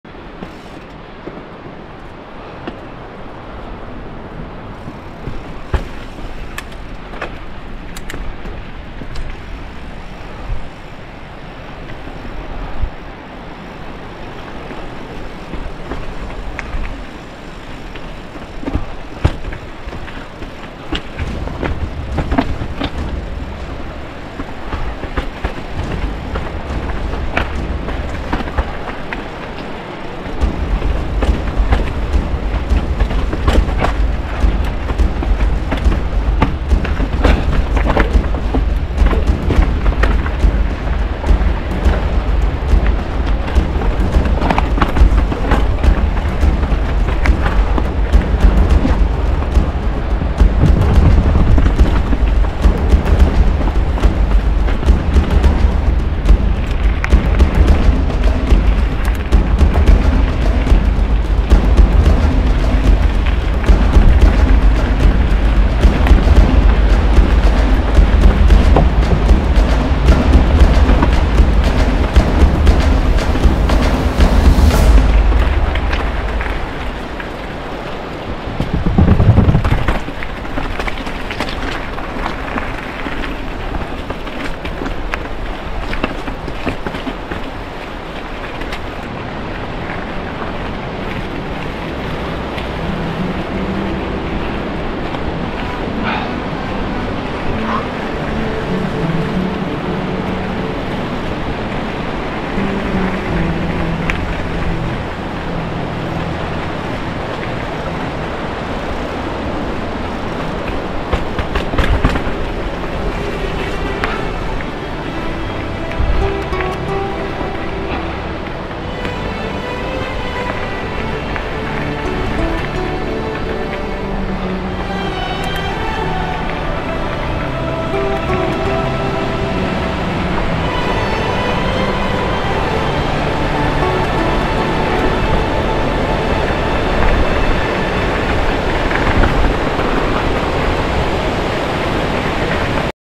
⚠ Sölden - Most dangerous MTB line ⚠
Most dangerous trail I have ever ridden in my life !!!